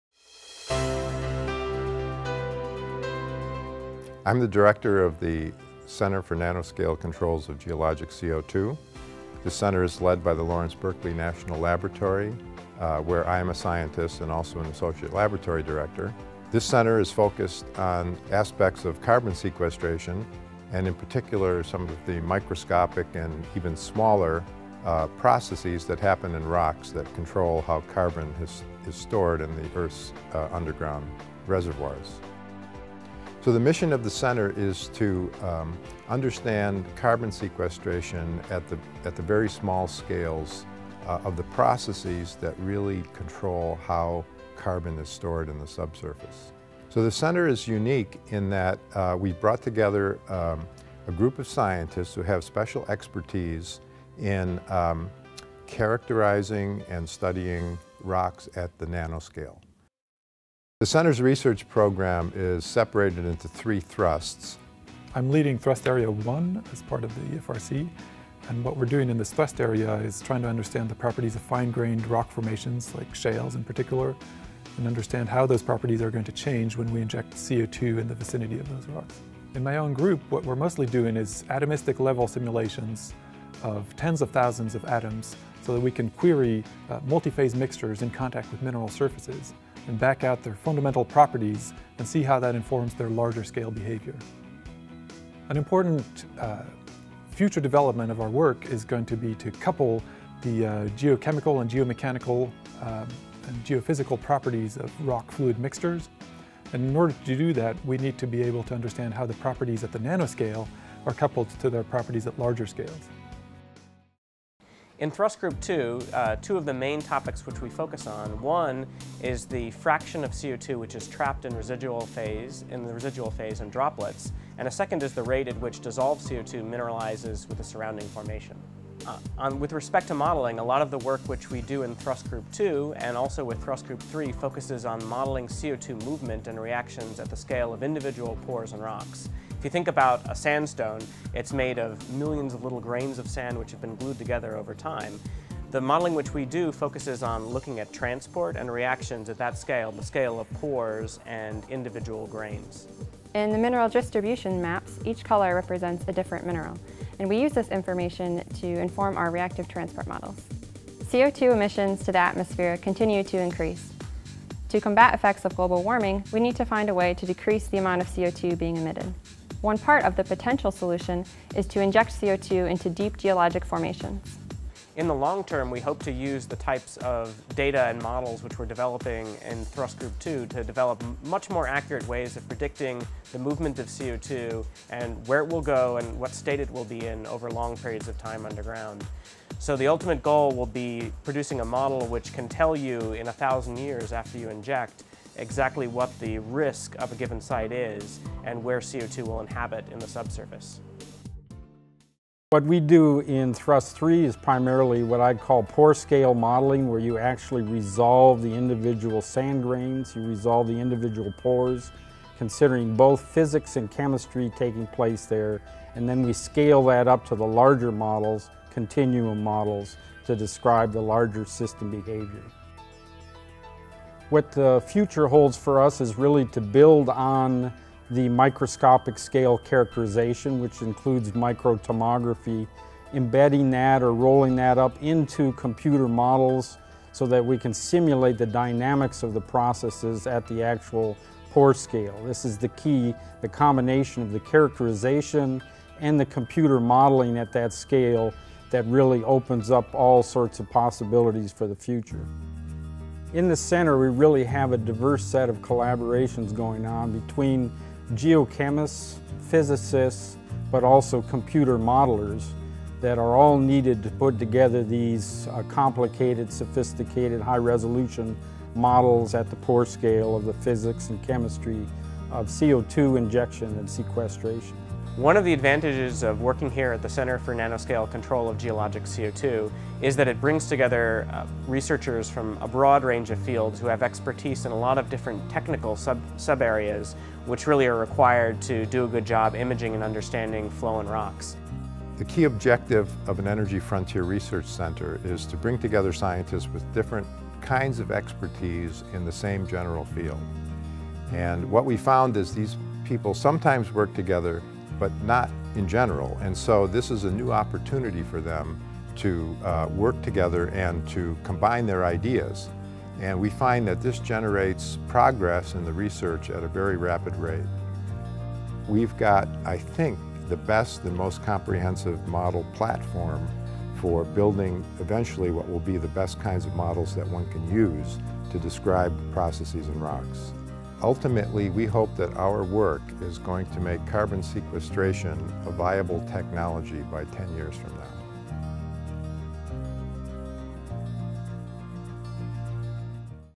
Center for the Nanoscale Control of Geologic CO2 - Lawrence Berkeley National Lab
The mission of the Center for the Nanoscale Control of Geologic CO2 is to enhance the performance and predictability of subsurface storage systems by understanding the molecular and nanoscale origins of CO2 trapping processes, and developing computational tools to translate to larger-scale systems. This DOE Energy Frontier Research Center (EFRC) is a collaborative effort led by Lawrence Berkeley National Laboratory (LBNL), the Oak Ridge National Laboratory (ORNL), Ohio State University, Purdue University, Stanford University, and Washington University in St. Louis.

To learn more about the unique collaboration happening at the center, watch this GSA TV video.